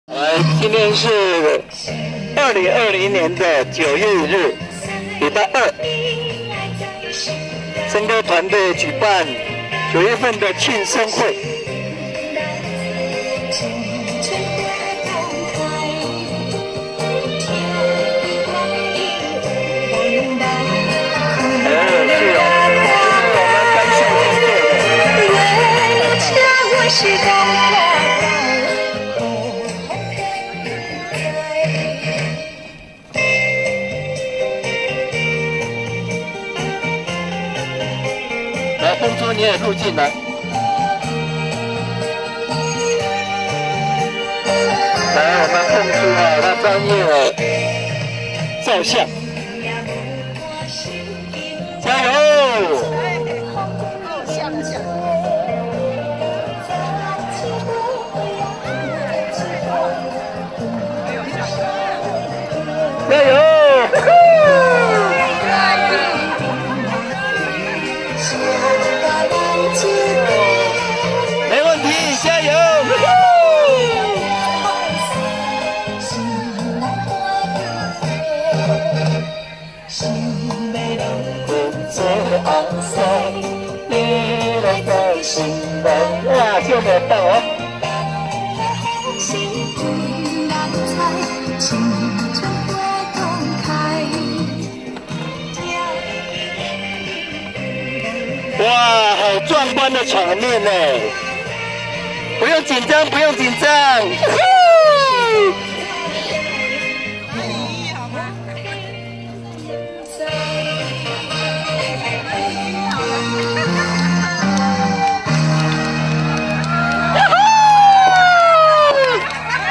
森哥團隊“快樂廣場舞群😊”舉辦慶生早餐會/2020/9/1
森哥團隊在竹南運動公園大廣場舉辦109年9月份慶生早餐會